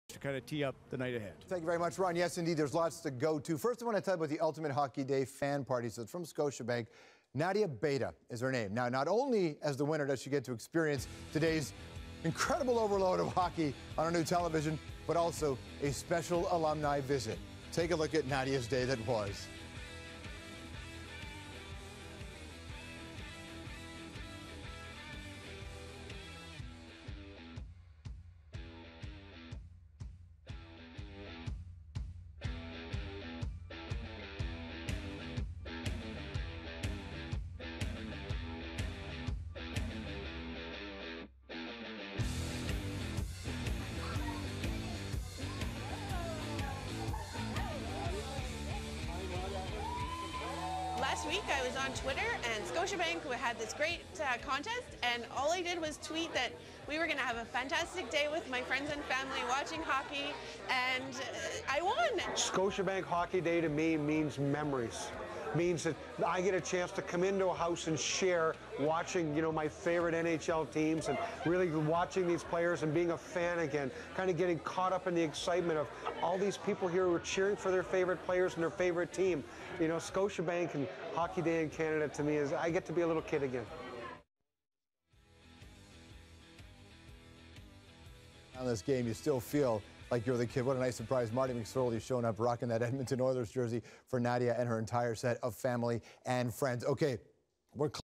Ultimate My HockeyDay Fan Party Highlights
Nadia Bayda and her family won the Ultimate My HockeyDay Fan Party contest which included a new big screen TV to watch Scotiabank Hockey Day in Canada and a surprise visit from NHL Alum Marty McSorley!